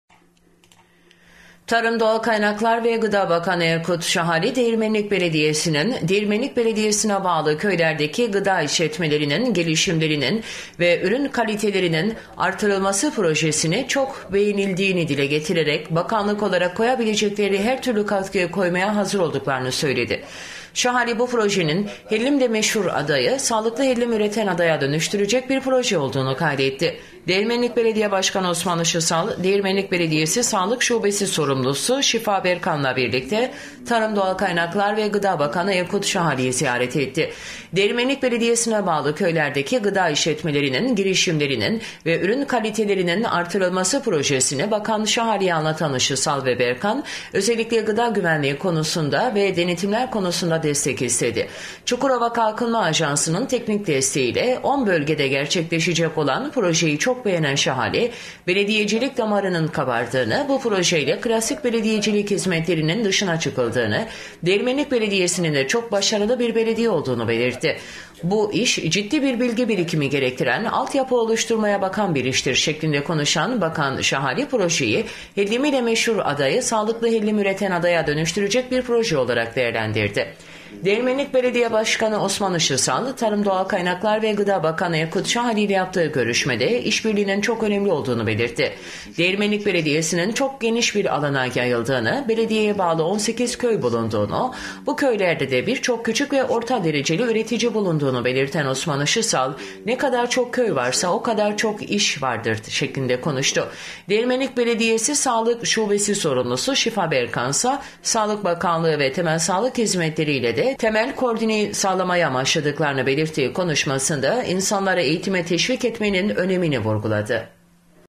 ŞAHALİ: HER TÜRLÜ KATKIYI KOYARIZ
Tarım, Doğal Kaynaklar ve Gıda Bakanı Erkut Şahali, Değirmenlik Belediyesi’nin “Değirmenlik Belediyesi’ne Bağlı Köylerdeki Gıda İşletmeleri’nin Gelişimleri’nin ve Ürün Kalitelerinin Arttırılması” projesini çok beğendiğini dile getirerek, bakanlık olarak koyabilecekleri her türlü katkıyı koymaya hazır olduklarını söyledi. Şahali, bu projenin, “hellimi ile meşhur Aya’yı, sağlıklı hellim üreten Aya’ya dönüştürecek bir proje” olduğunu kaydetti.  Değirmenlik Belediye Başkanı Osman Işısal, Değirmenlik Belediyesi Sağlık Şubesi Sorumlusu Şifa Berkan ile birlikte Tarım, Doğal Kaynaklar ve Gıda Bakanı Erkut Şahali’yi ziyaret etti.  “Değirmenlik Belediyesi’ne Bağlı Köylerdeki Gıda İşletmeleri’nin Gelişimleri’nin ve Ürün Kalitelerinin Arttırılması” projesini Bakan Şahali’ye anlatan Işısal ve Berkan, özellikle gıda güvenliği konusunda ve denetimler konusunda destek istedi. Çukurova Kalkınma Ajansı’nın teknik desteği ile 10 bölgede gerçekleşecek olan projeyi çok beğenen Şahali, belediyecilik damarının kabardığını, bu proje ile klasik belediyecilik hizmetlerinin dışına çıkıldığını, Değirmenlik Belediyesi’nin de çok başarılı bir belediye olduğunu belirtti. “Bu iş, ciddi bir bilgi birikimi gerektiren, alt yapı oluşturmaya bakan bir iştir” şeklinde konuşan Bakan Erkut Şahali, projeyi, “hellimi ile meşhur Aya’yı, sağlıklı hellim üreten Aya’ya dönüştürecek bir proje” olarak değerlendirdi.
Değirmenlik Belediyesi’nin çok geniş bir alana yayıldığını, belediyeye bağlı 18 köy bulunduğunu, bu köylerde de birçok küçük ve orta dereceli üretici bulunduğunu belirten Osman Işısal, “ne kadar çok köy varsa, o kadar da çok iş vardır” şeklinde konuştu.
BERKAN: “EĞİTİME TEŞVİK ÖNEMLİ”
Değirmenlik Belediyesi Sağlık Şubesi Sorumlusu Şifa Berkan ise, Sağlık Bakanlığı ve Temel Sağlık Hizmetleri ile de temel koordineyi sağlamayı amaçladıklarını belirttiği konuşmasında, insanları eğitime teşvik etmenin önemini vurguladı.